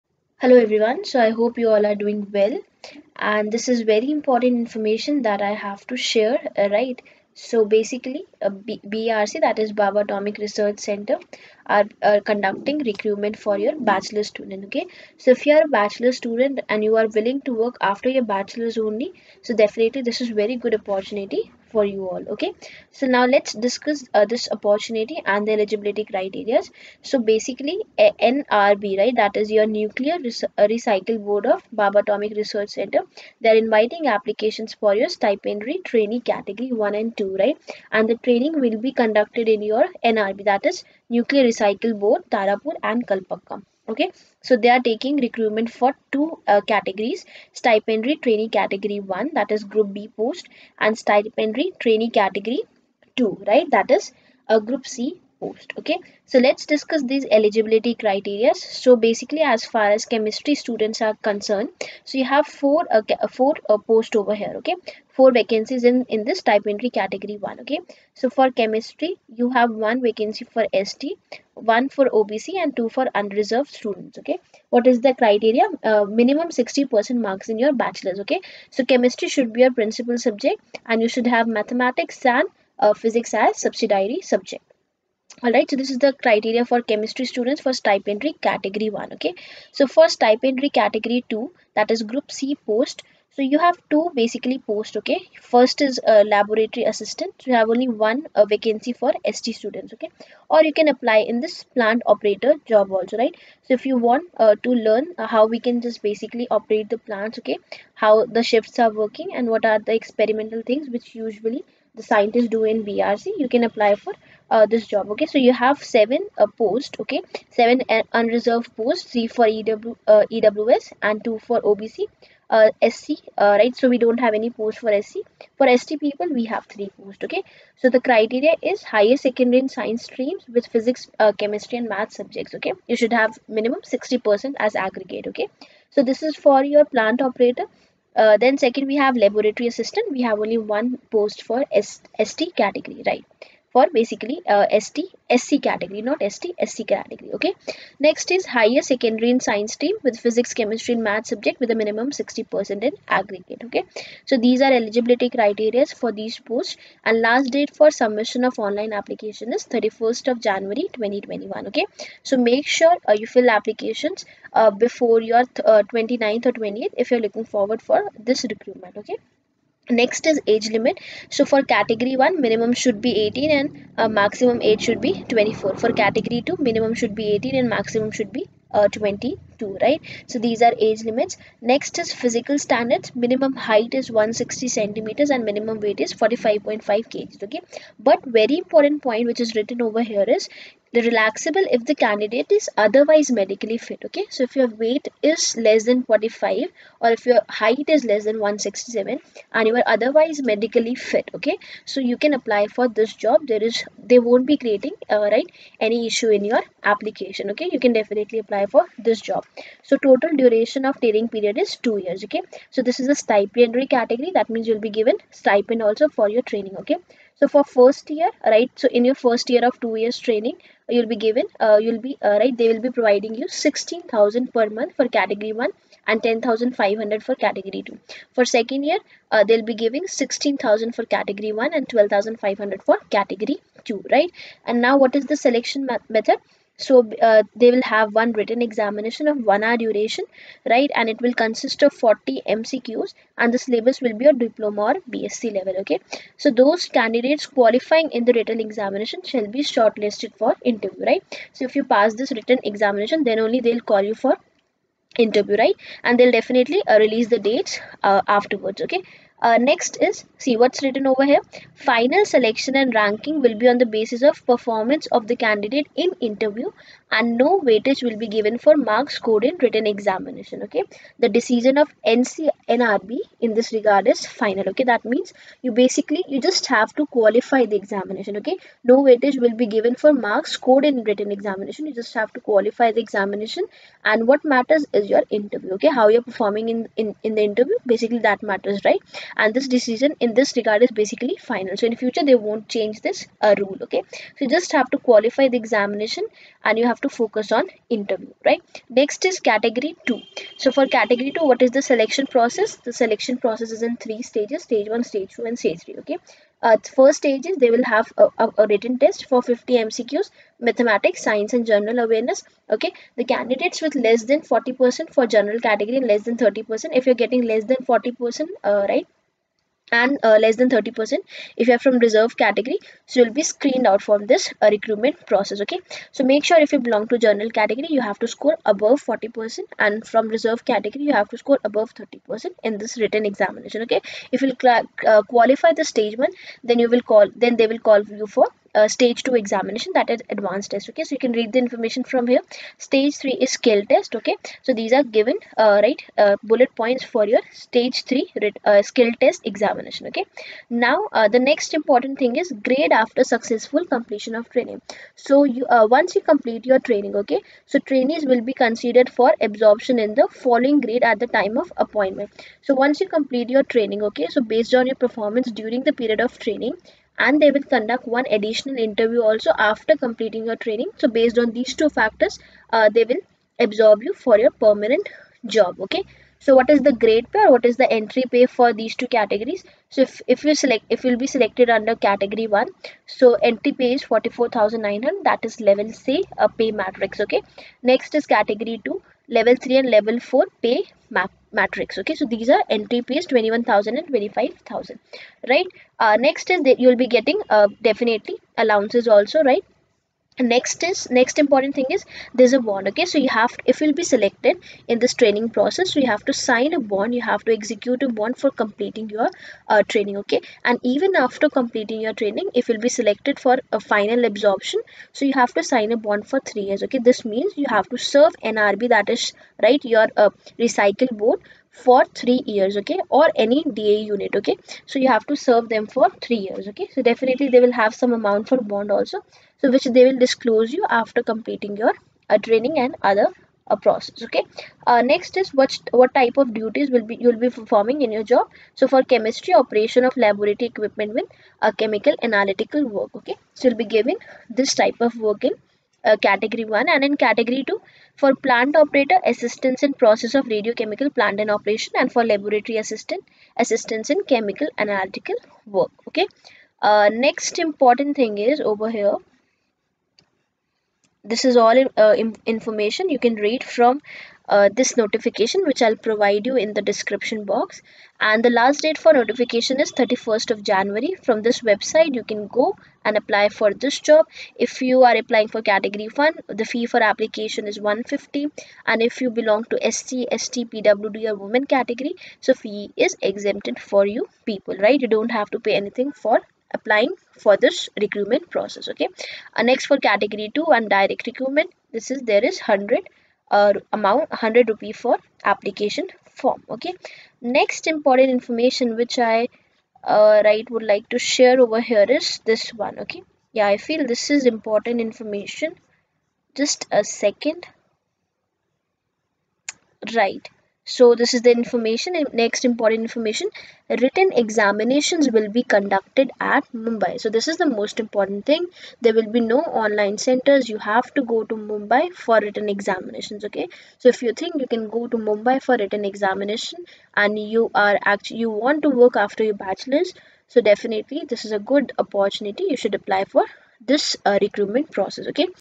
BARC Chemistry Jobs | B.Sc. Eligible | Group B & Group C | Level 7 Pay matrix
Excellent opportunity for B.Sc. chemistry students who want a government job in chemistry field.